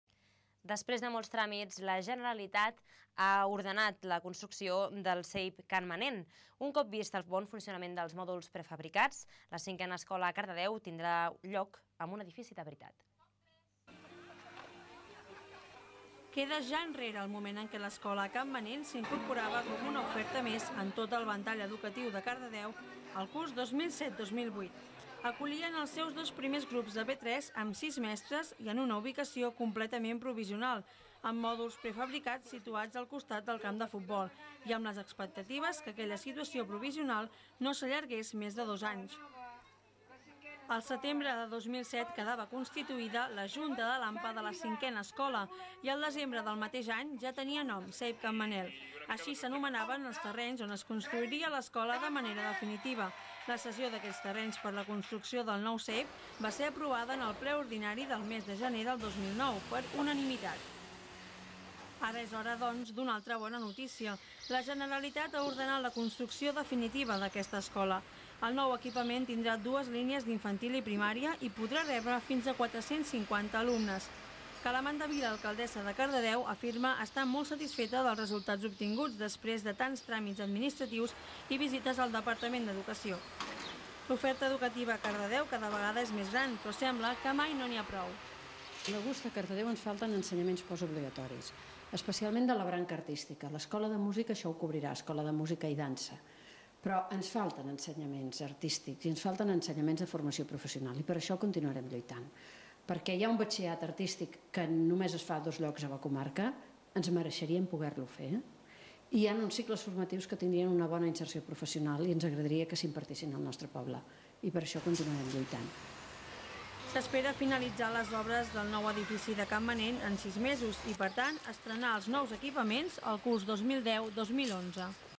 02 CAN MANENT JA TÉ DATA
Després de molts tràmits la Generalitat ha ordenat la construcció del CEIP Can Manent. Un cop vist el bon funcionament en mòduls prefabricats, la cinquena escola de Cardedeu tindrà un edifici de veritat.